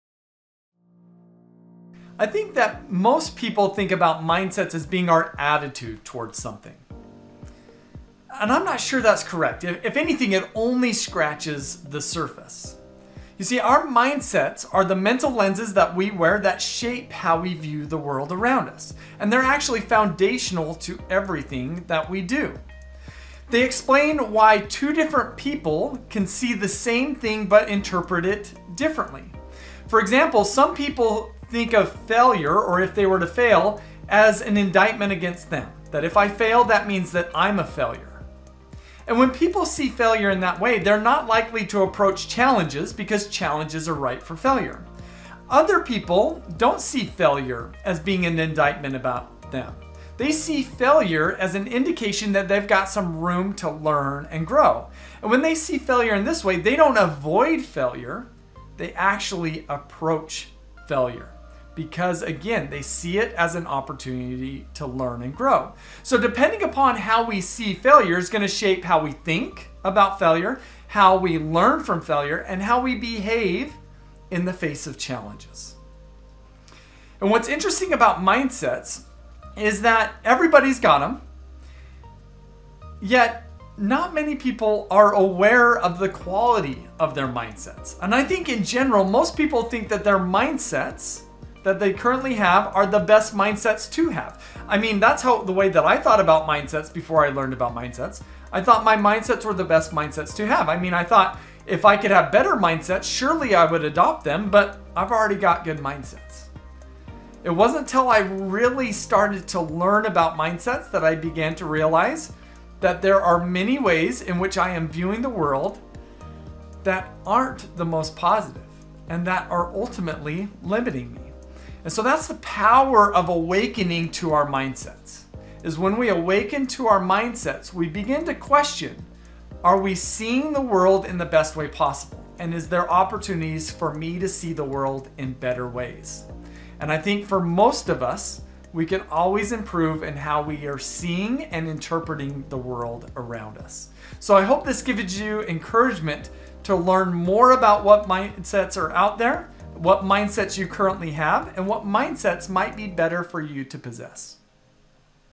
What Are Mindsets?
People often think that our mindsets are our attitudes toward something.

That is a very short-sighted perspective, as our mindsets are so much more than that.

Want to get clear on what our mindsets really are? Watch this short video.